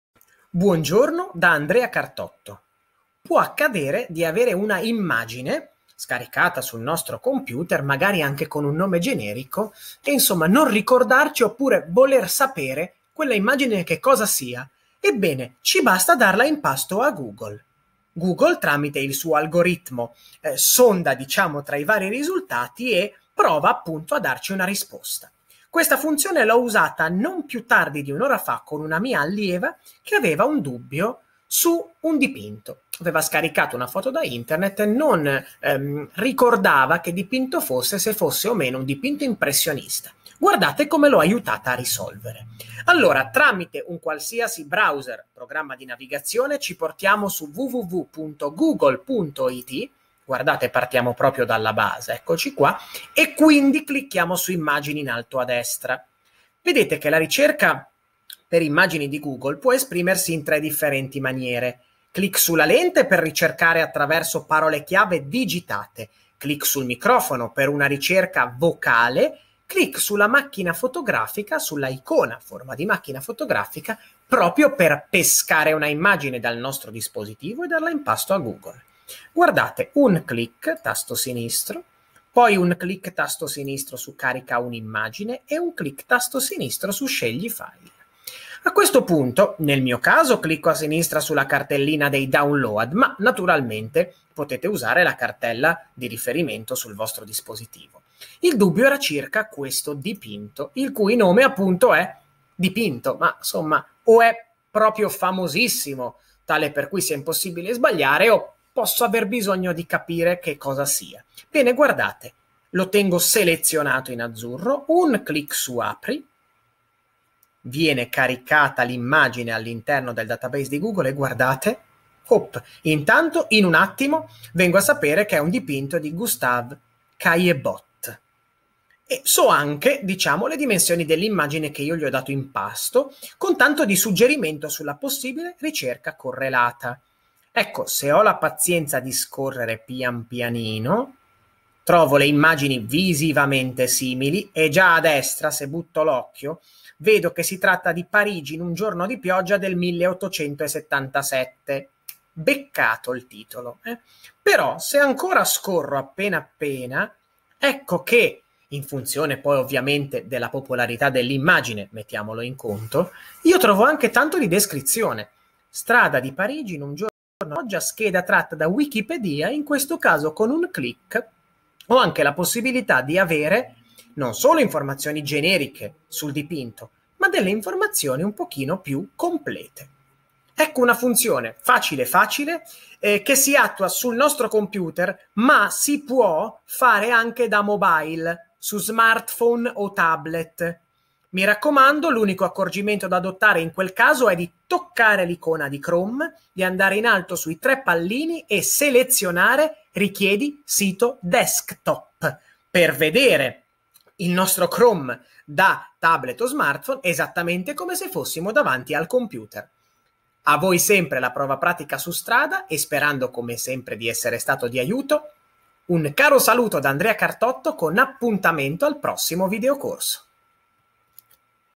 Andrea Cartotto - Come fare una ricerca inversa per immagini con Google
La ricerca inversa per immagini ci consente di partire da una immagine in nostro possesso, di cui non abbiamo informazioni o un nome file definito, per ottenere direttamente da Google una descrizione e maggiori dettagli. Utile ad esempio, come nel caso che descrivo passo passo nel videocorso, per risalire dall'immagine di un dipinto al titolo dell'opera e al suo autore. P.s. Si può fare anche da smartphone e tablet, con un piccolo accorgimento che vi racconto sempre qui.